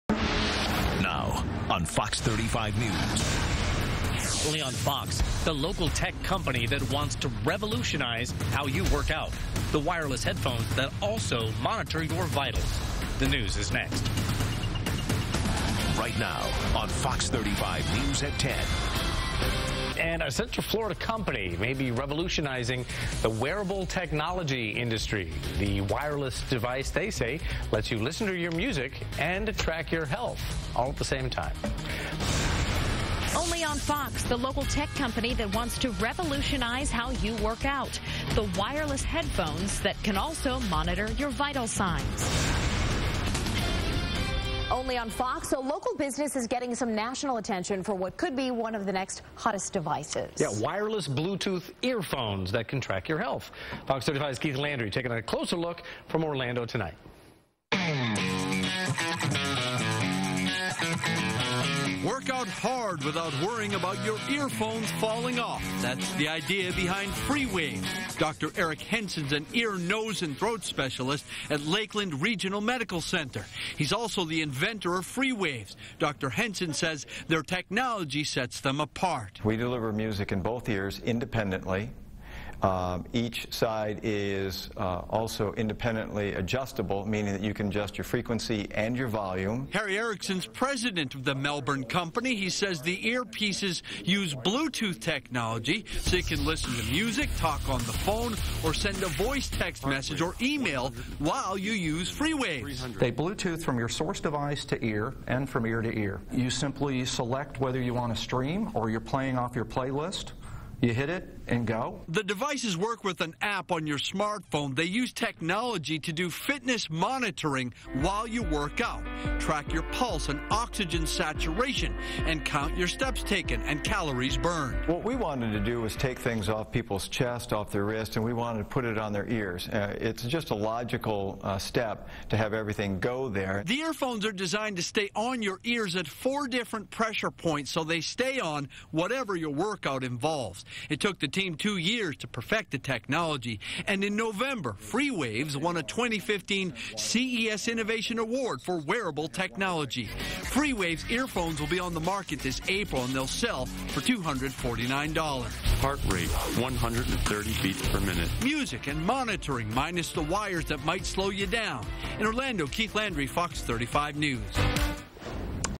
FreeWavz - WOFL Fox 35
WOFL Fox 35 Coverage:

Local tech company, FreeWavz,want to revolutionize the way you workout - the wireless headphones that also measure your vitals!

Listen to music & track your vitals. Music and monitoring, minus the wires!